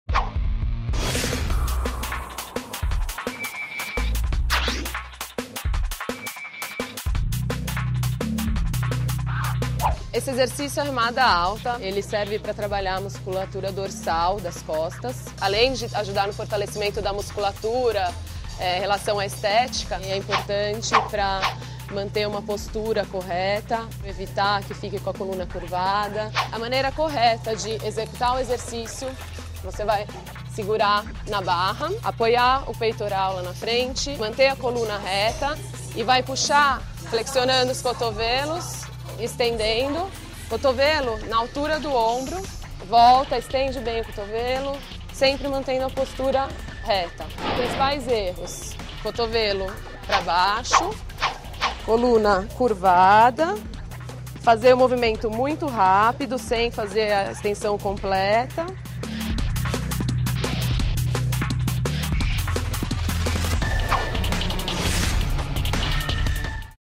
Faça a coisa certa: remada alta
A remada é o melhor exercício para trabalhar as costas. A ginástica fortalece a região e garante uma postura de princesa.